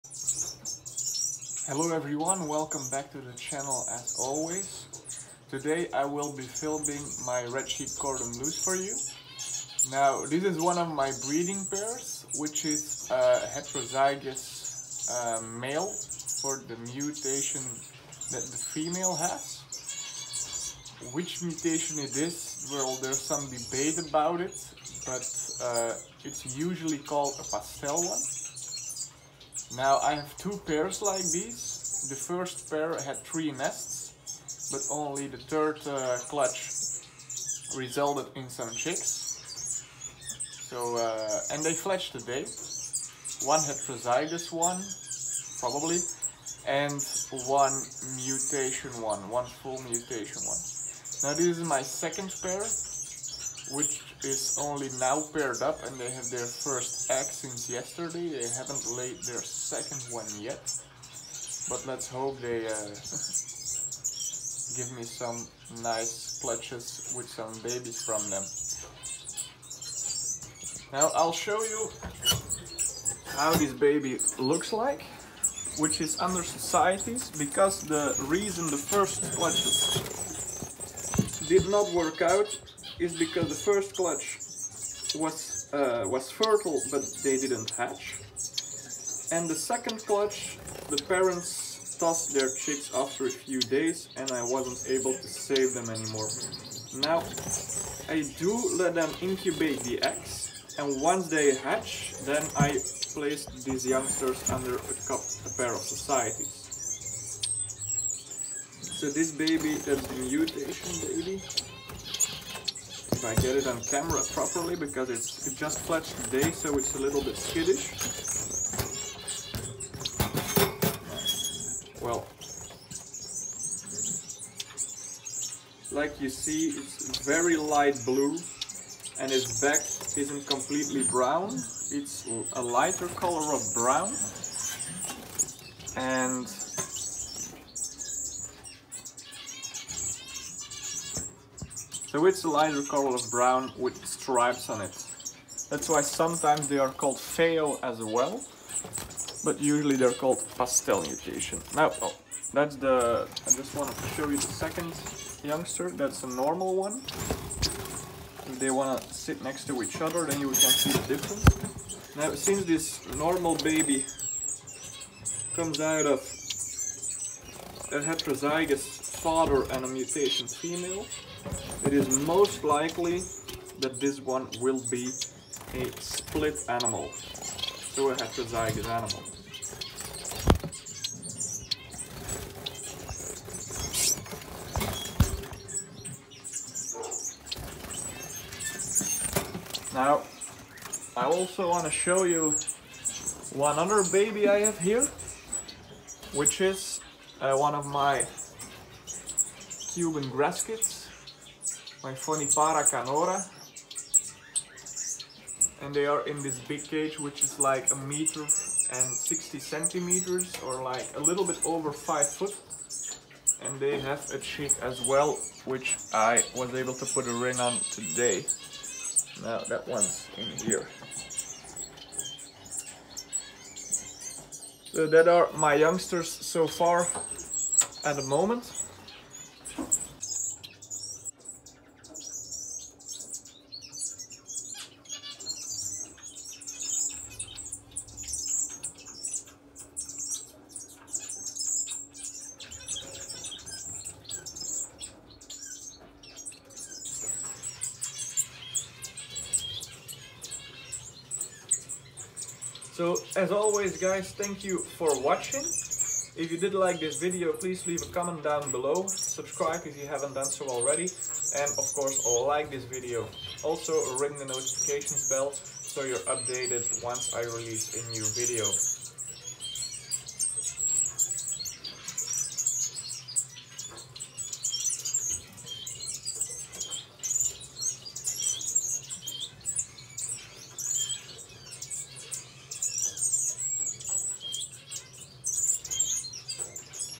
Baby mutation red-cheeked cordon-bleu
Today my first pastel cordon-bleu chick fledged. This is the first I've ever bred myself.